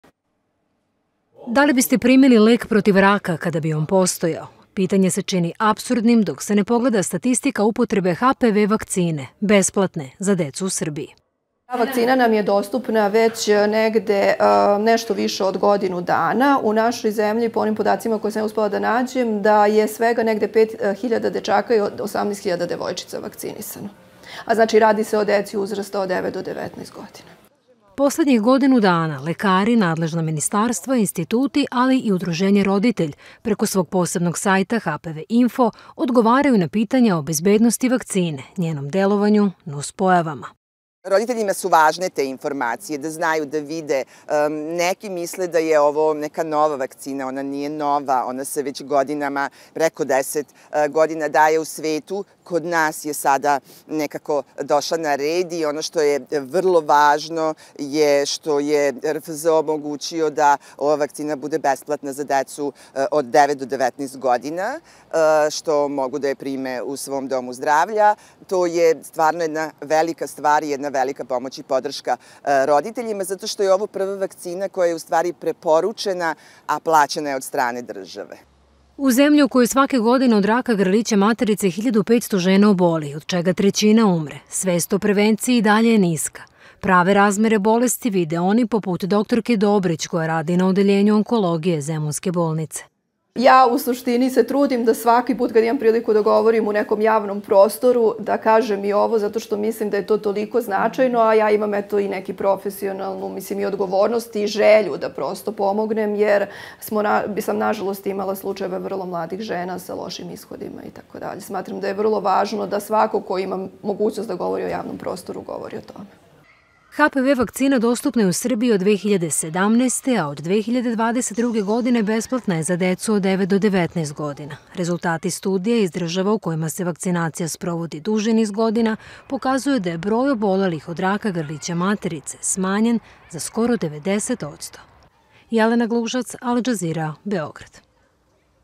U Srbiji slab odaziv na besplatnu HPV vakcinaciju
Iako je od prošle godine besplatno za djevojčice i dječake od 9 do 19 godina, HPV vakcina, koja štiti od teških bolesti poput raka grlića maternice, do sada je u Srbiji primilo manje od 4 posto djece.

Zbog čega je odaziv skroman, istražila je Jelena Glušac.